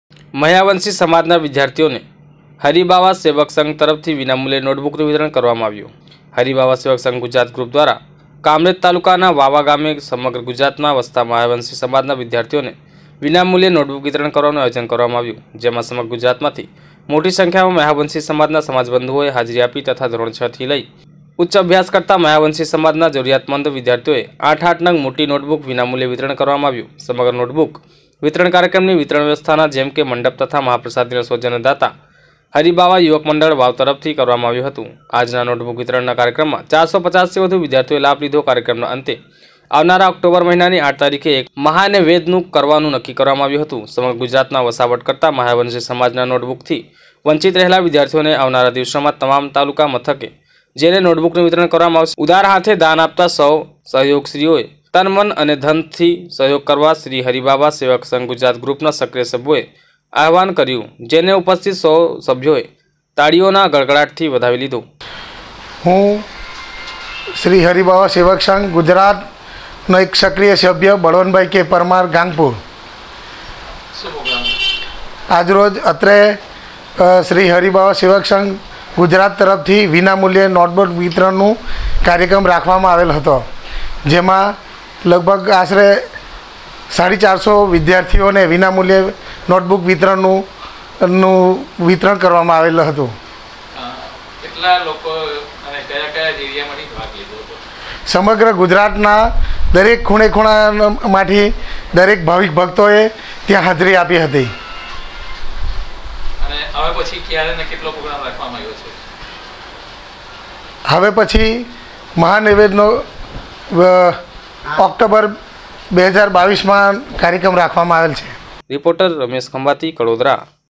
માહ્યાવંશી સમાજના વિદ્યાર્થીઓને શ્રી હરિબાવા સેવક સંઘ તરફથી વિનામૂલ્યે...|TV9INDIA GUJARATI
માહ્યાવંશી સમાજના વિદ્યાર્થીઓને શ્રી હરિબાવા સેવક સંઘ તરફથી વિનામૂલ્યે નોટબુકનું વિતરણ કરવામાં આવ્યું

શ્રી હરિબાવા સેવક સંઘ- ગુજરાત* ગ્રુપ  ધ્વારા કામરેજ તાલુકાના વાવ ગામે સમગ્ર  ગુજરાતમાં વસતા માહયાવંશી સમાજના  વિદ્યાર્થીઓને વિના મુલ્યે નોટબુક  વિતરણ કરવાનુ આયોજન કરવામાં આવ્યું જેમા  સમગ્ર ગુજરાતમાંથી મોટી સંખ્યામાં  માહયાવંશી સમાજના સમાજબંધુઓ એ હાજરી આપી તથા  ધોરણ 6 થી લઈ ઉચ્ચ અભ્યાસ કરતા માહયાવંશી સમાજના જરૂરીયાતમંદ વિદ્યાર્થીઓને  8 - 8 નંગ મોટી નોટબુક વિના મુલ્યે વિતરણ કરવામા આવ્યુ* સમગ્ર નોટબુક વિતરણ કાર્યક્રમ ની વિતરણ વ્યવસ્થા ના જેમકે મંડપ તથા મહા પ્રસાદી ના સૌજન્ય દાંતા શ્રી હરીબાવા યુવક મંડળ વાવ તરફથી કરવામાં આવ્યું હતું આજના નોટબુક  વિતરણ ના કાર્યકમ મા 450 થી વધુ વિદ્યાર્થીઓએ લાભ લીધો કાર્યક્રમ ના અંતે આવનારા ઓક્ટોબર મહિનાની 8 તારીખે એક મહાનૈવેધ નુ કરવાનુ નક્કી કરવામાં આવ્યું હતું સમગ્રગુજરાતમાં વસવાટ કરતામાહ્યાવંશી સમાજના નોટ બુકથી વંચિત રહેલા વિદ્યાર્થીઓને આવનારા દિવસોમાં તમામ તાલુકા મથકે જેને નોટબુક નું વિતરણ કરવામાં આવશે ઉદાર હાથે દાન આપતા સૌ સહયોગીશ્રીઓ ને તન મન અને ધન થી સહયોગ કરવા શ્રી હરીબાવા સેવક સંધ ગુજરાત ગૃપ ના સક્રિય સભ્યો એ આહવાન કર્યું જેને ઉપસ્થિત સૌ સભ્યોએ એ તાડીઓના ગડગડાટ થી વધાવી લીધુ